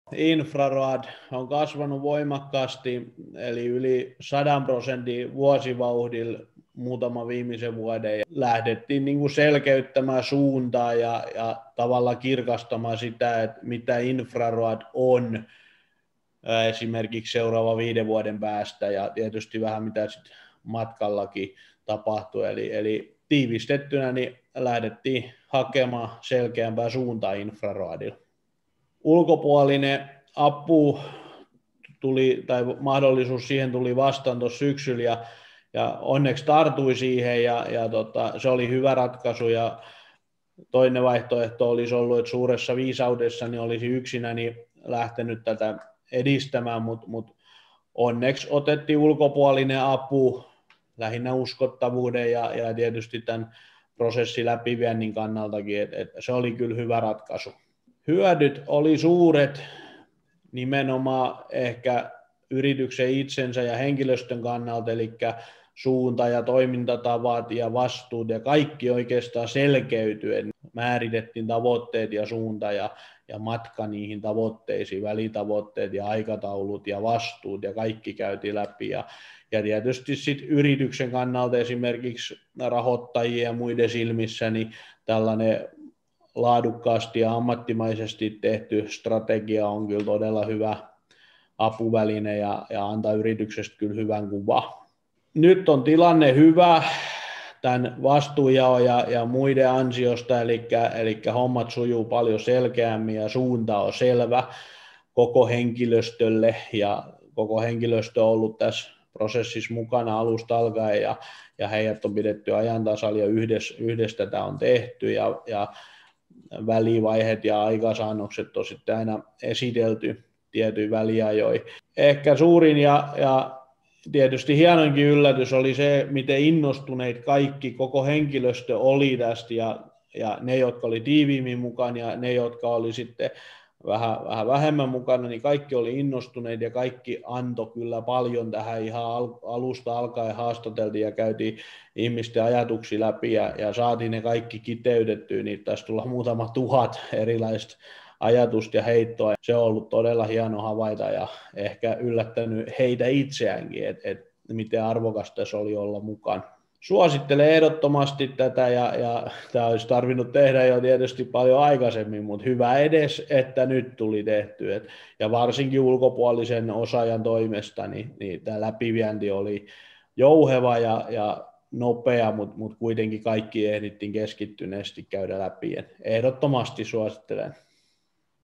Infraroad, Markus Lahti
Tässä videossa esitellään meidän seuraavaa asiakastamme strategiaprosessista, jossa puhuu Markus Lahti Infraroadista.